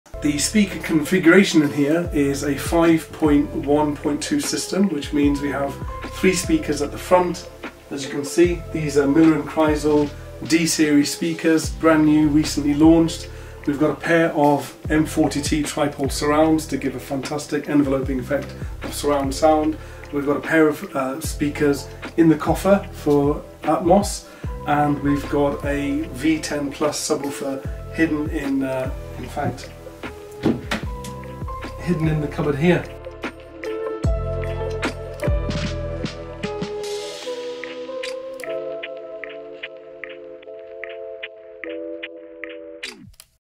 Home Cinema Speakers - NEW MILLER AND KREISEL D-SERIES
Creating the ultimate cinema room experience with perfect speaker placement from M&K Sound! From the centre channel to surround speakers, every element is carefully positioned for mind-blowing audio immersion.

Get ready to feel every rumble and hear every whisper. 🎶🎬

I would first like to invite my viewers to come to visit us at my other locations.